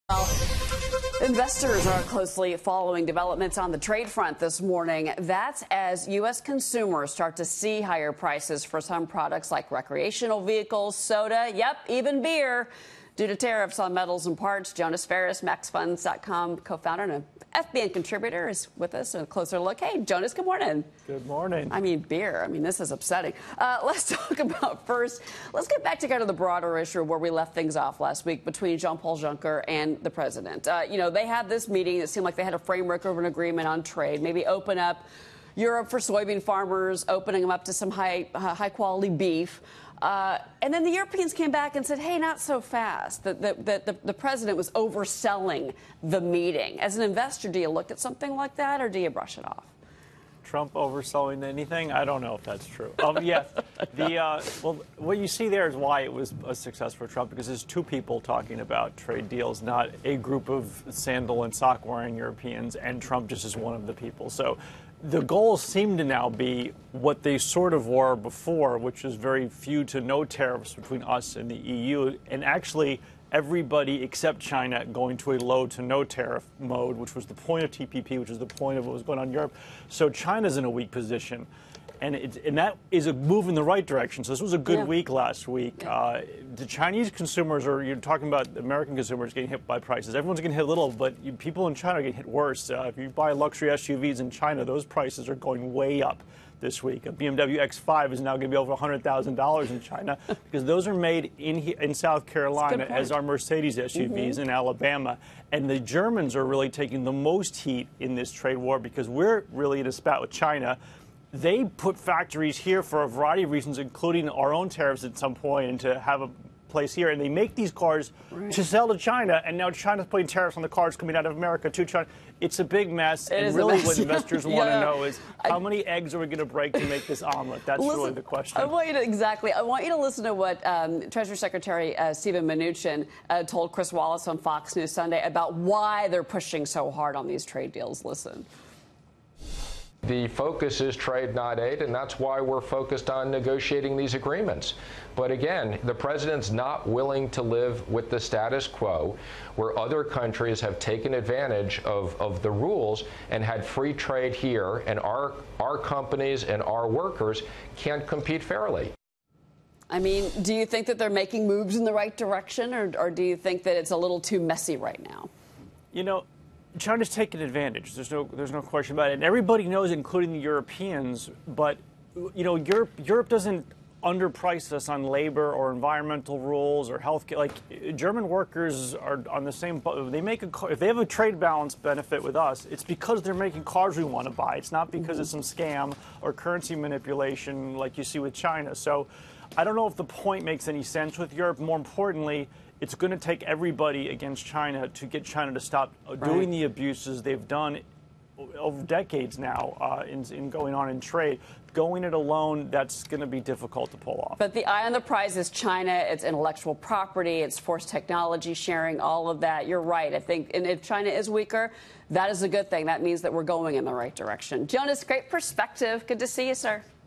China in a weak position in trade talks?
Fox News contributor Jonas Max Ferris on the market impact from trade concerns.